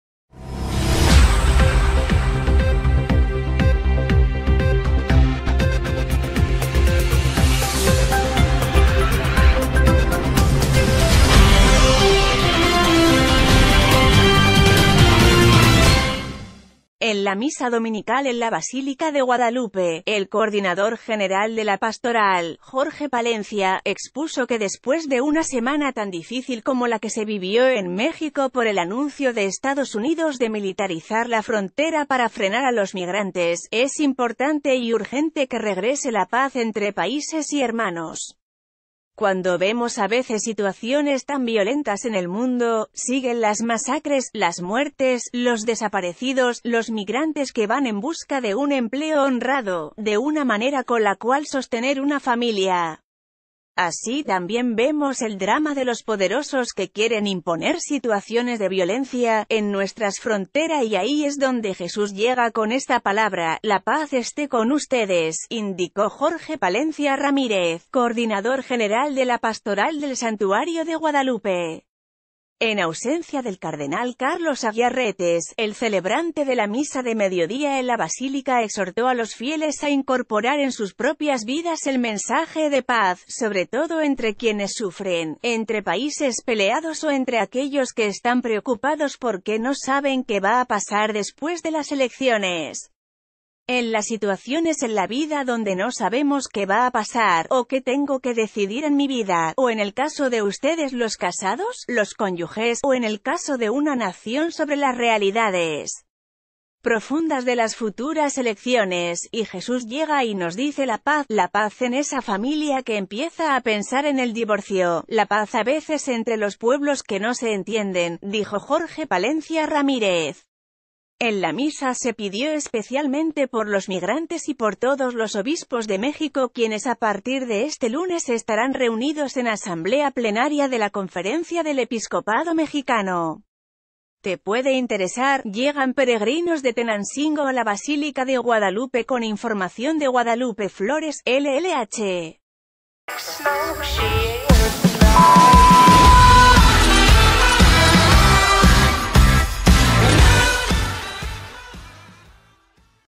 Piden por migrantes durante misa dominical en la Basílica - Televisa News
En la misa dominical en la Basílica de Guadalupe, el coordinador general de la Pastoral, Jorge Palencia, expuso que después de una semana tan difícil como la que se vivió en México por el anuncio de Estados Unidos de militarizar la frontera para frenar a los migrantes, es importante y urgente que re...

{hashtag}